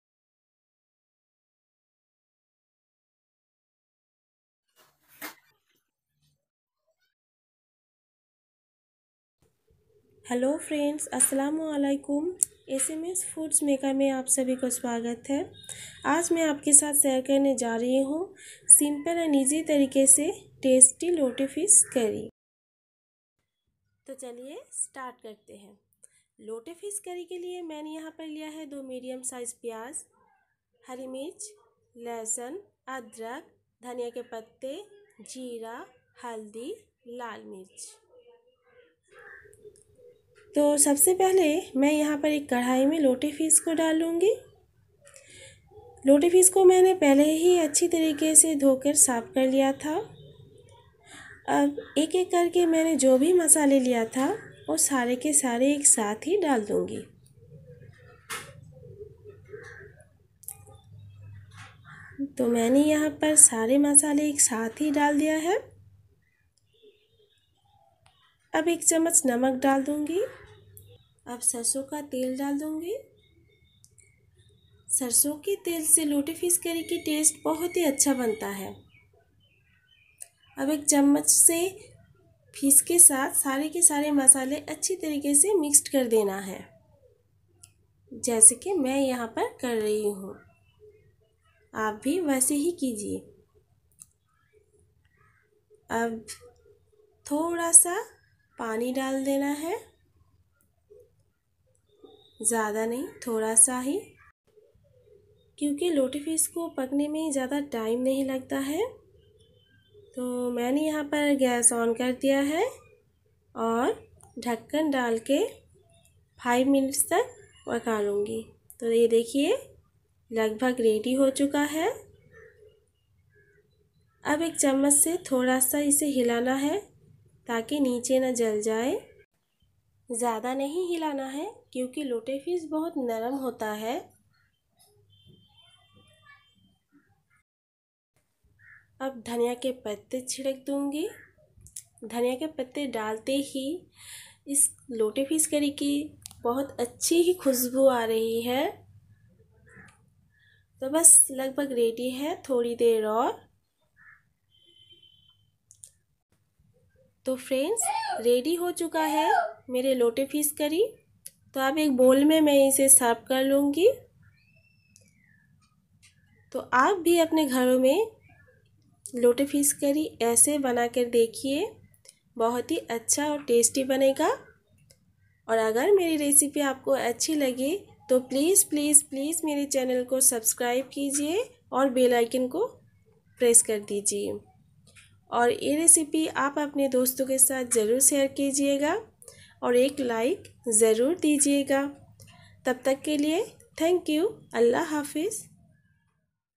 Sabse aasan aur majedar lotte fish curry.
Lotte fish carry banane ka saral trick. Very easy very simple recipe fish curry.lotte fish curry,lotte fish curry Kolkata style,